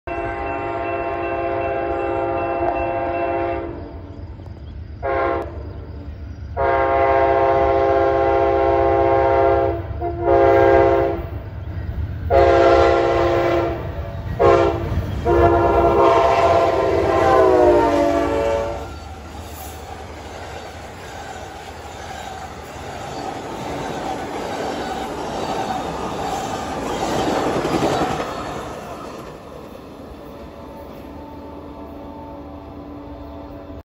An Extremely fast AMTK 49 through Chesterton, IN! 6/10/22
Amtrak 49 flies thru Chesterton, IN on June 10, 2022 at 8:43 AM CST.

Consists of:
11 cars